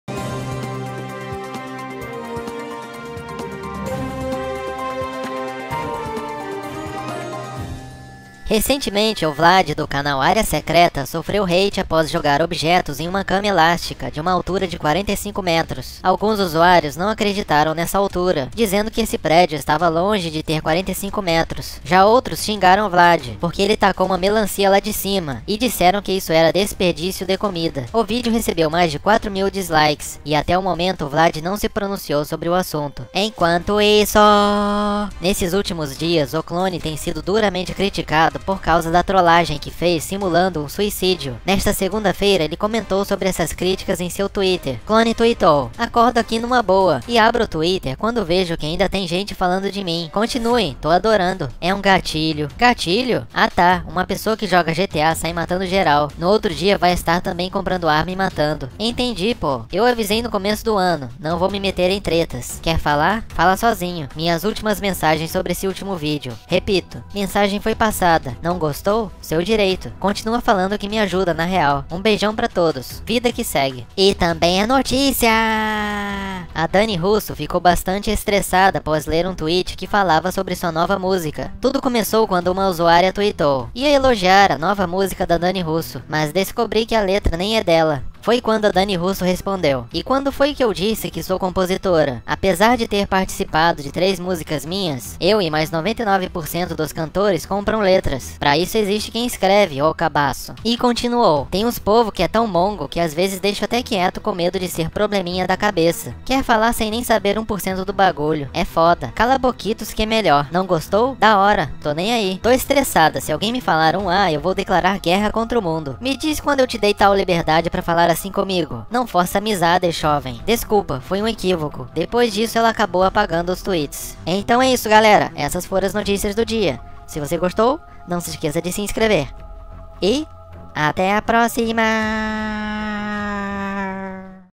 Dani Russo se revolta depois de levar alfinetada, Clone se defende
Dani Russo ficou indignada com tweet, Clone respondeu críticas